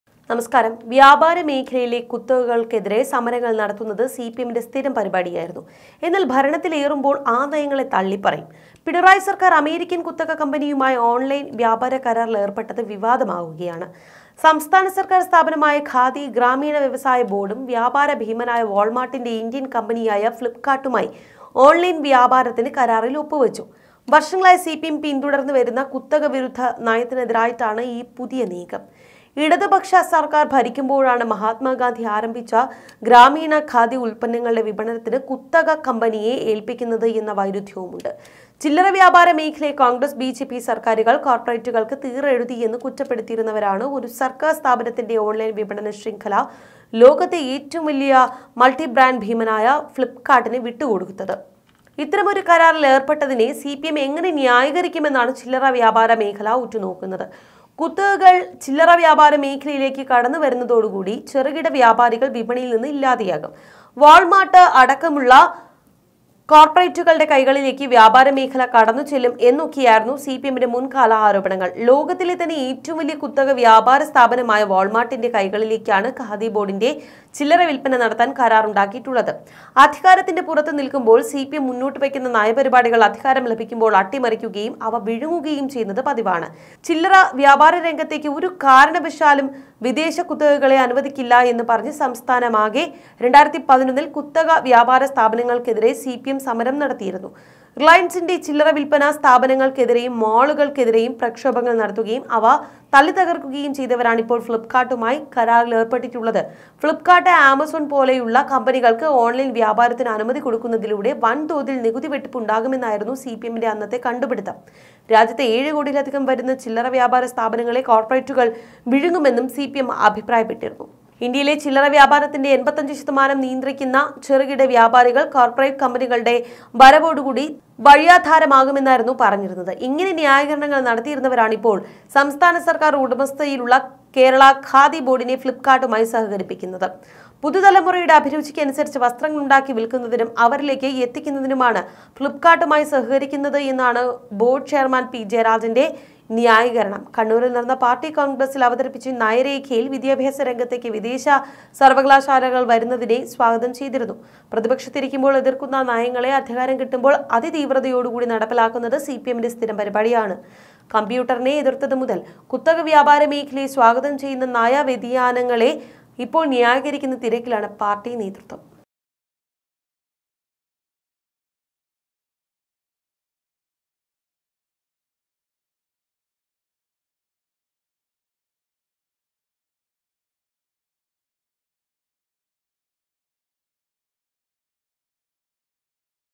ഖാദി ബോര്‍ഡും ഫ്ളിപ്പ് കാര്‍ട്ടും തമ്മില്‍ ഓണ്‍ലൈന്‍ വില്പനയ്ക്ക് ധാരണ I Kadi Board
ഗാന്ധിജി തുടങ്ങിയ ഖാദി ഉൽപന്നങ്ങളുടെ വിൽപ്പന ആഗോള ഭീമന് തീറെഴുതി ഇടത് സർക്കാർ; കുത്തക വ്യാപാരത്തിനെതിരെ സമരം നടത്തിയത് പഴങ്കഥ; ഖാദി ബോർഡും ഫ്‌ളിപ്പ് കാർട്ടും തമ്മിൽ ഓൺലൈൻ വില്പന കരാറിലെത്തി; പണ്ട് പറഞ്ഞതെല്ലാം വിഴുങ്ങുന്നത് സിപിഎം പതിവ് പരിപാടി